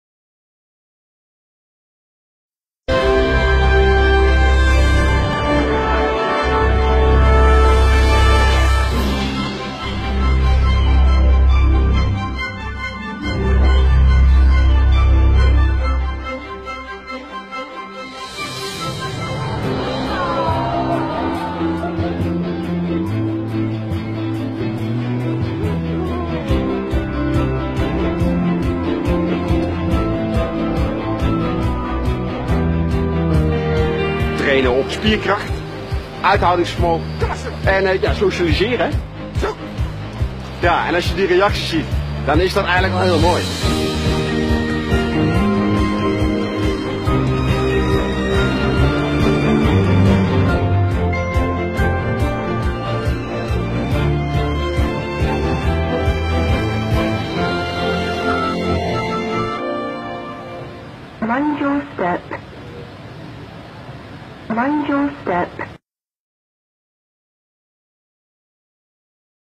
The cutest airline employee.
a Dutch Airline (KLM) hires a canine to retrieve lost belongings of their passengers. And it works well for everyone.  The canine seems to be enjoying his job and the passengers are happy and grateful upon seeing their lost objects and the cuteness of the retriever/employee😍🥰😂.